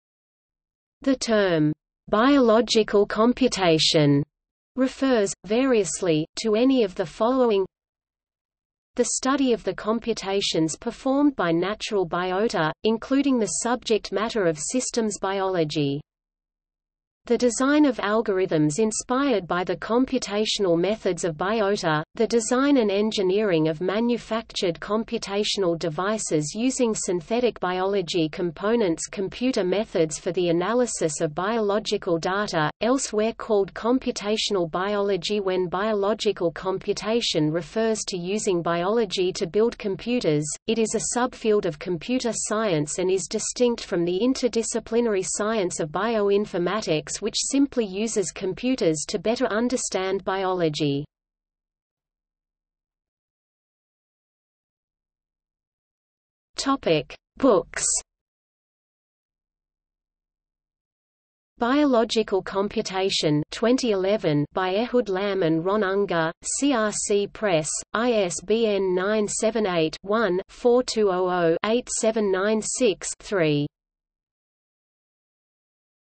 Biological computation | Wikipedia audio article
00:00:53 Books

- Socrates

SUMMARY
The term "biological computation" refers, variously, to any of the following:
- the study of the computations performed by natural biota, including the subject matter of systems biology.
- the design of algorithms inspired by the computational methods of biota - the design and engineering of manufactured computational devices using synthetic biology components- computer methods for the analysis of biological data, elsewhere called computational biologyWhen biological computation refers to using biology to build computers, it is a subfield of computer science and is distinct from the interdisciplinary science of bioinformatics which simply uses computers to better understand biology.